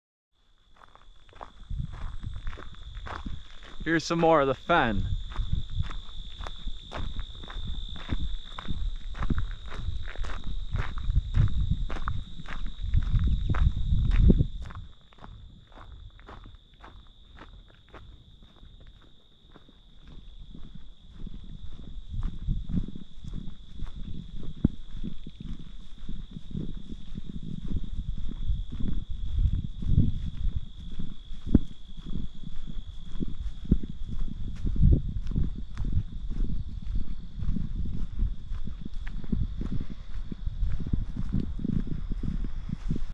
Veteran Acres & Sterne's Woods, IL - Sterne's Woods Fen #2 (Hike 360 VR Video)
Use the 360 video features to spin around and view this area. Before hiking in the midwest, I would generally ignore fields like this. Now, I better understand their significance and why these hiking oasis exist in the middle of farm land.